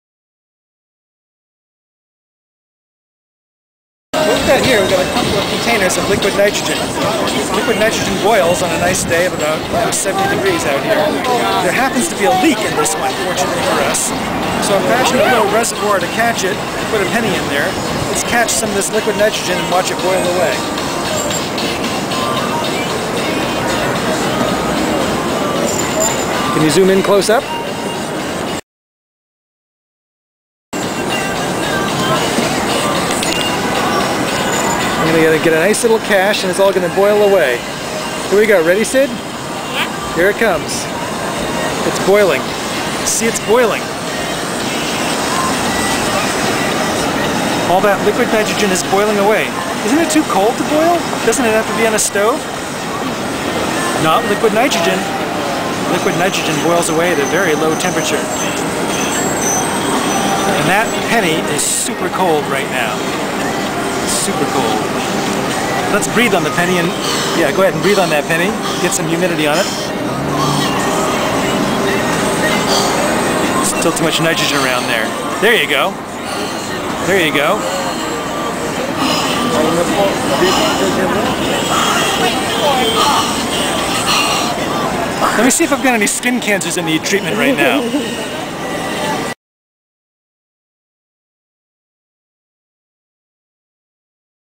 Gotham Streetcorner Cryogenics
It is said you can get anything in New York (at a price). On a street corner we found two canisters of liquid nitrogen to play with, for free!